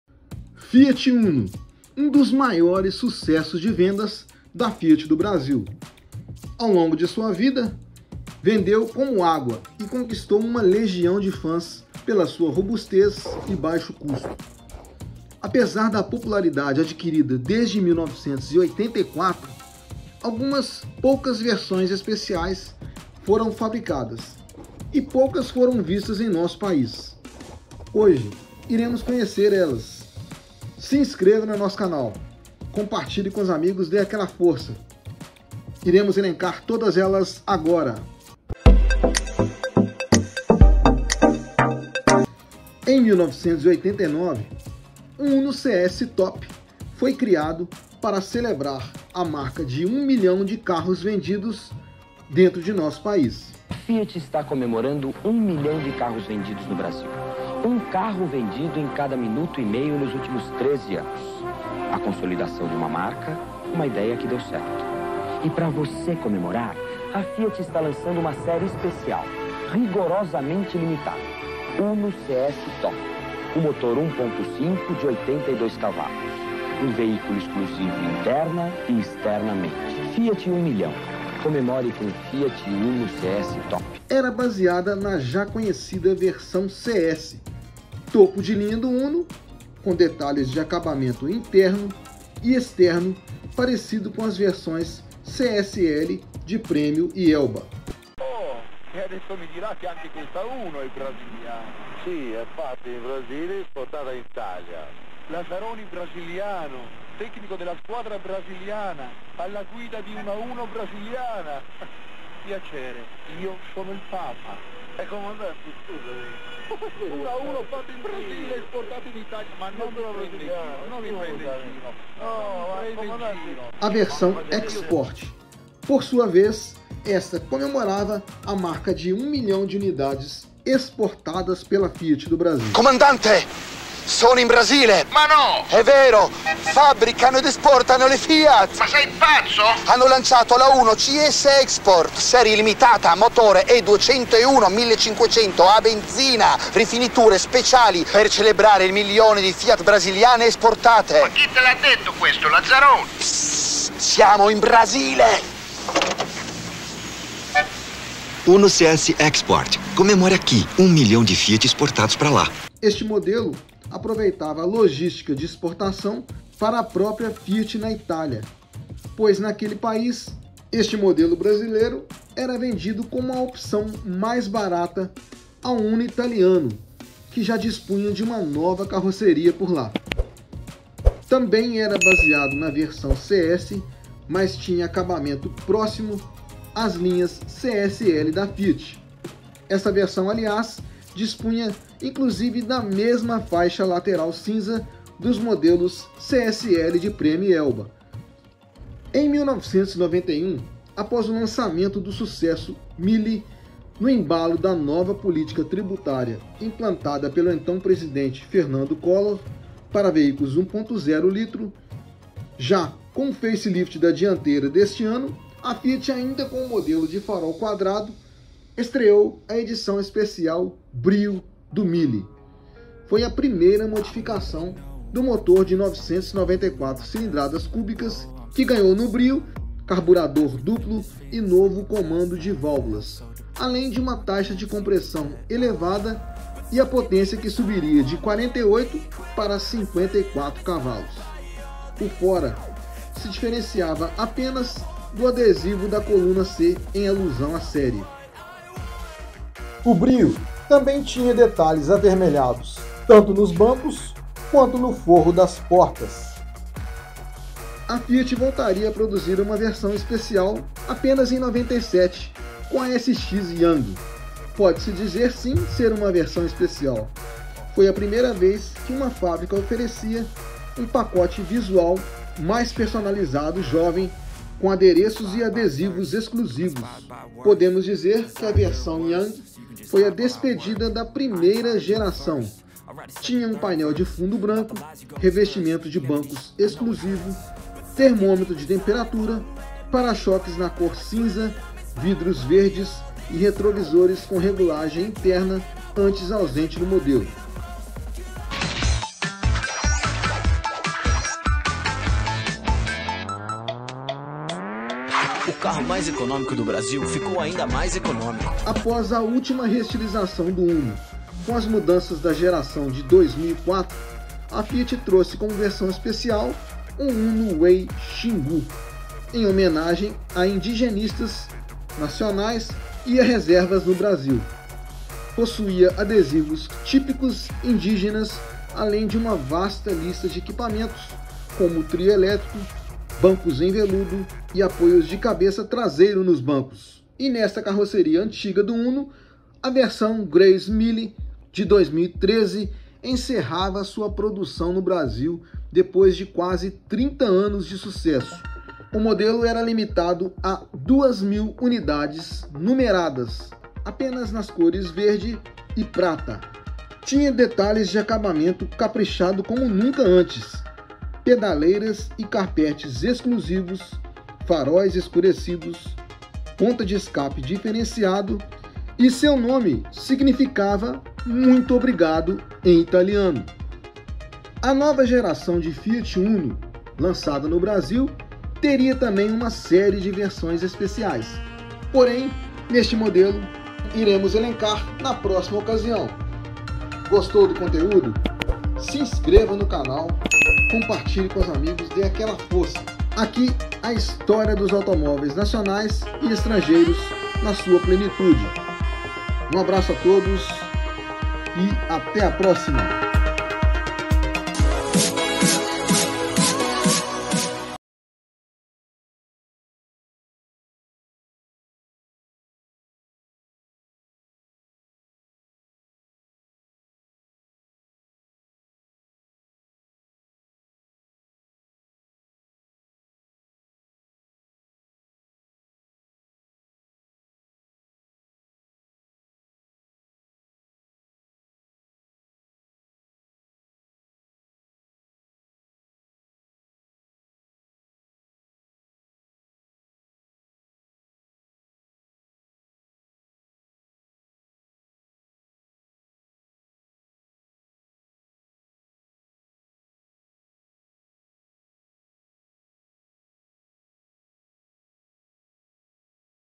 Mille Brio, CS Top, Grazie Mille entre outras... As VERSÕES ESPECIAIS do Fiat Uno no Brasil !
Documentário com as versões limitadas especiais do campeão de vendas da Fiat no Brasil: O Fiat Uno de primeira geração!